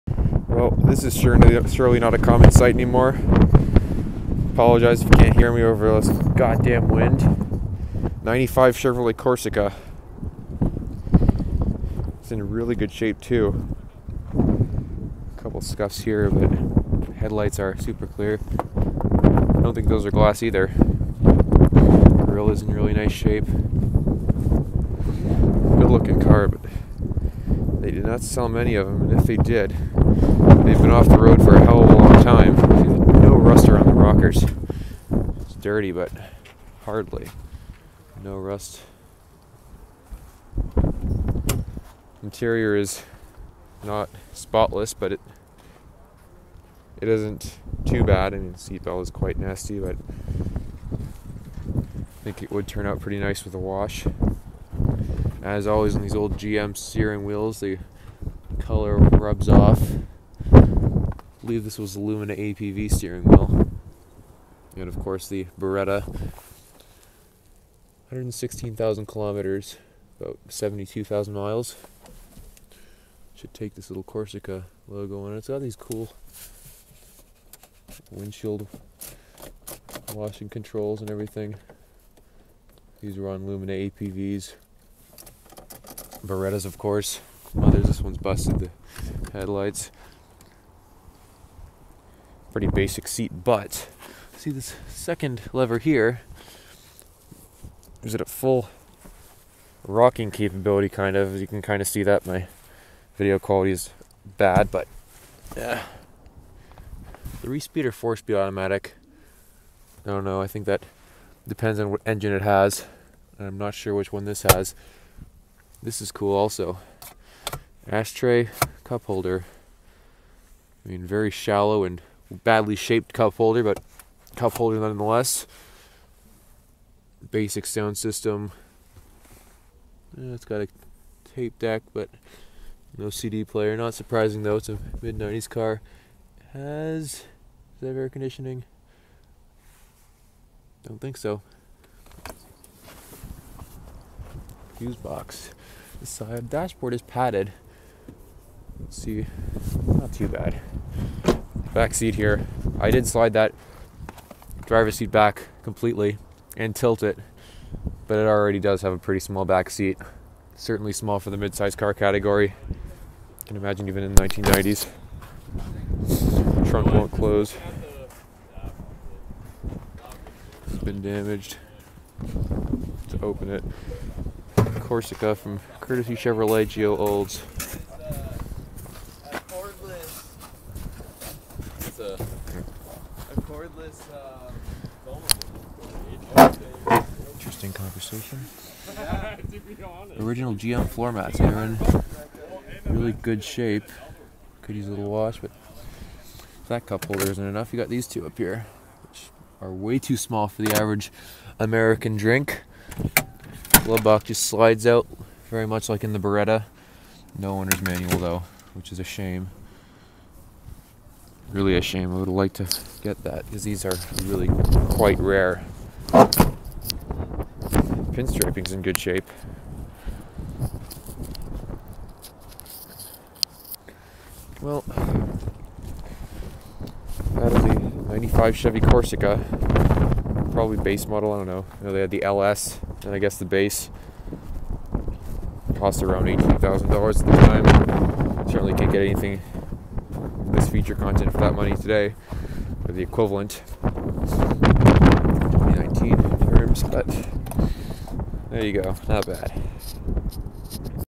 Junkyard Tour: 1995 Chevrolet Corsica w/112k km
Quick tour of a rust-free, low mileage 1995 Chevrolet Corsica